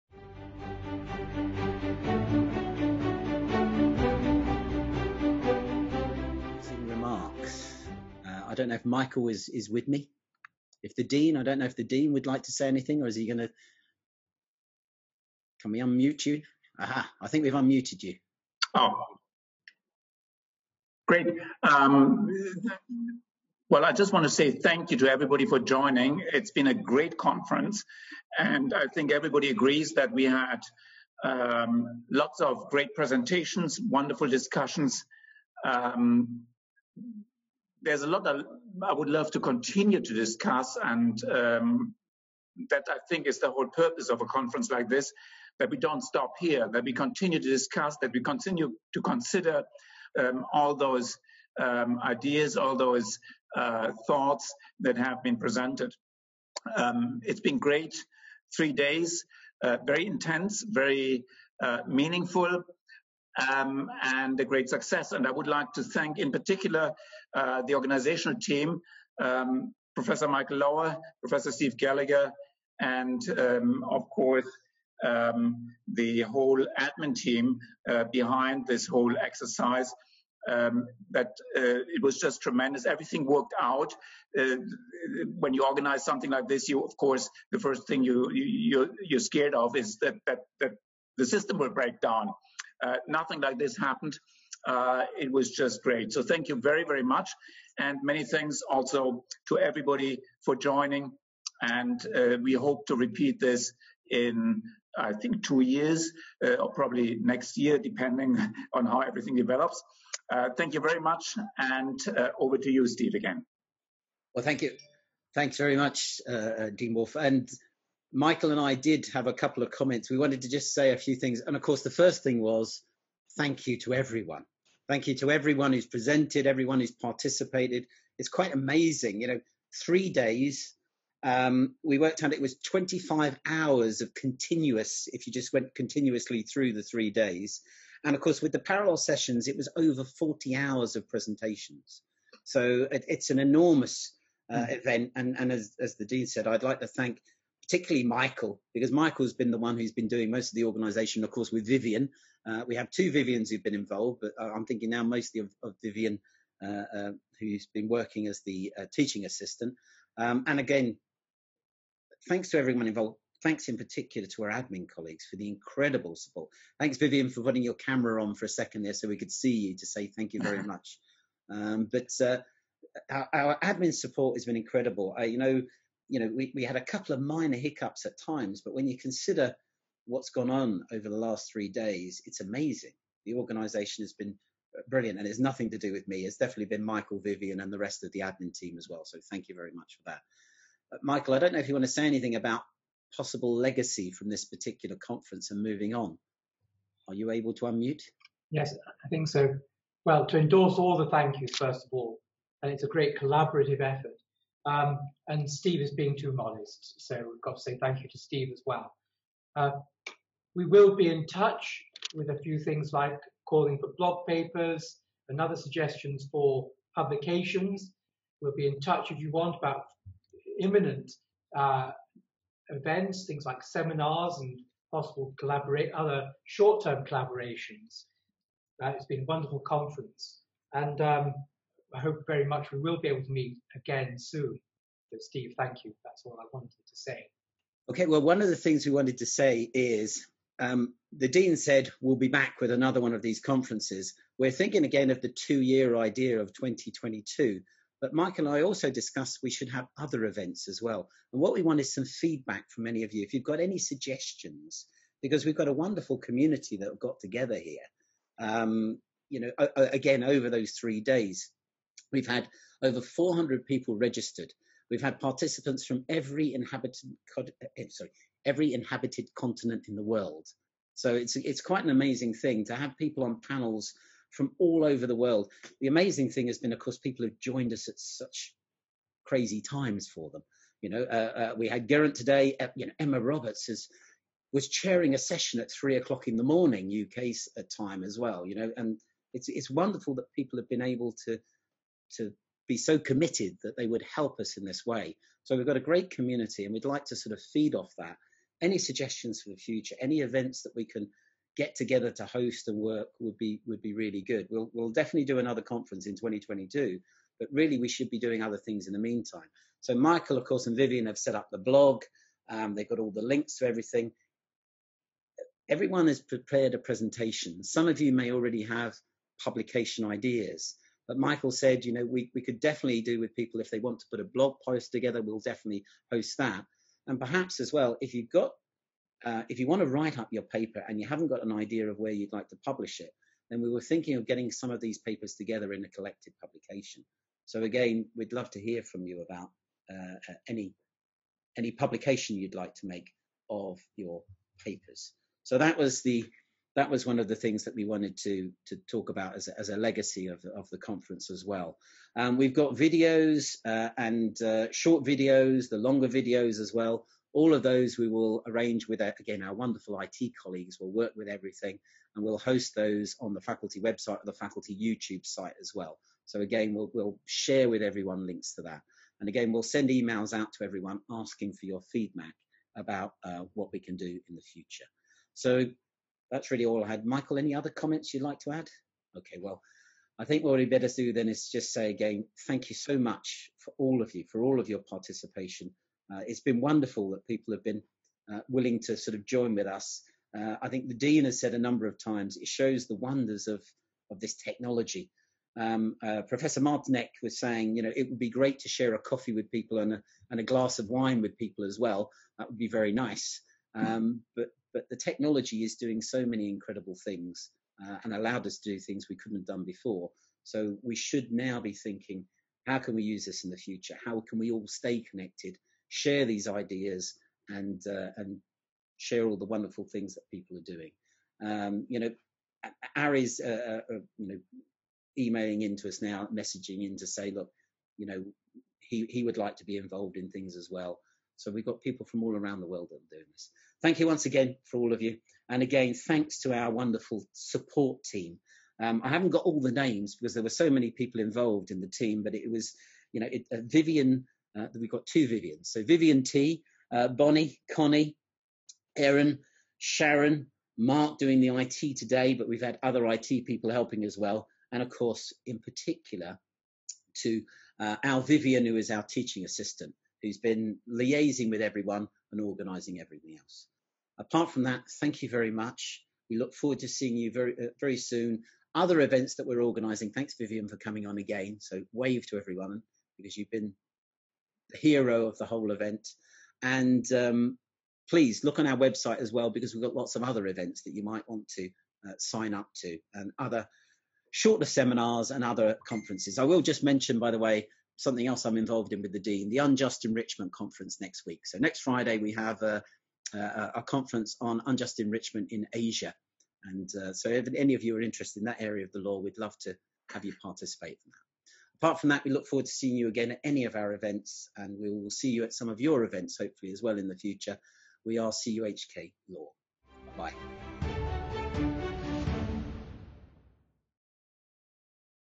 Directions in Legal Education 2020 - Day 3 Closing Remarks
CUHK LAW Directions in Legal Education 2020 - Online Conference on Teaching & Learning in Law

Presentation Title:
Closing Remarks

Date:
18-20 June 2020